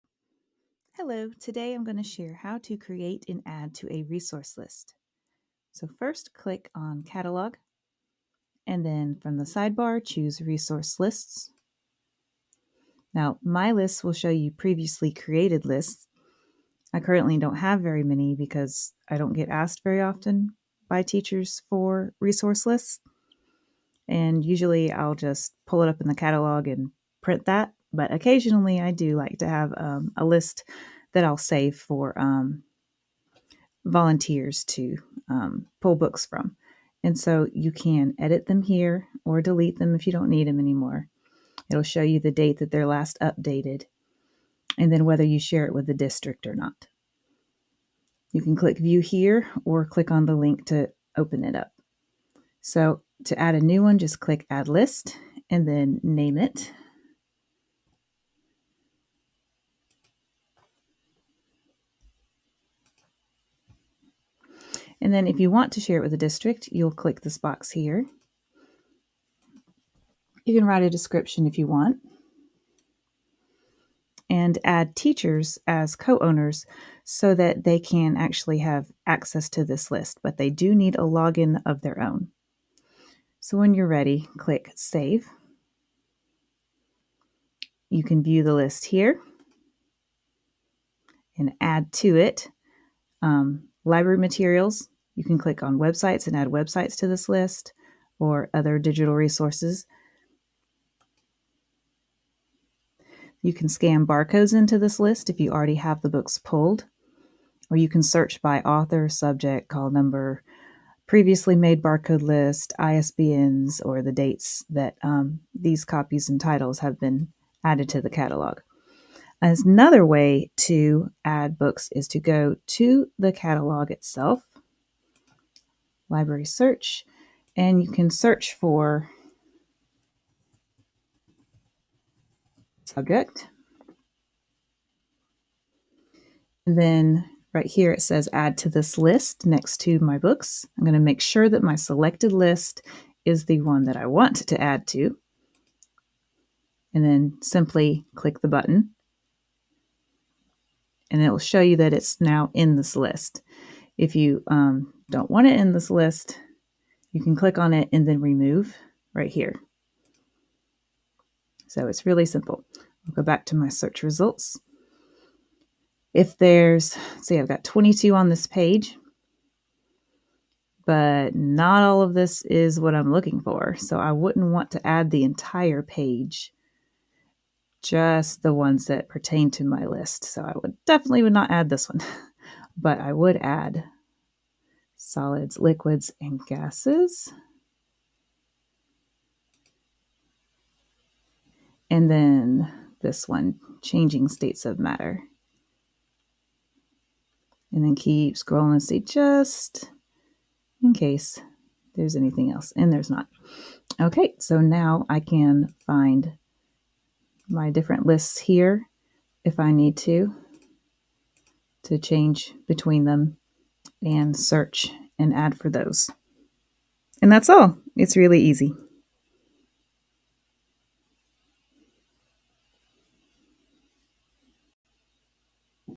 Follett Destiny - Create and Add to Resource Lists
Learn how to create a new resource list & add new materials to old and new lists.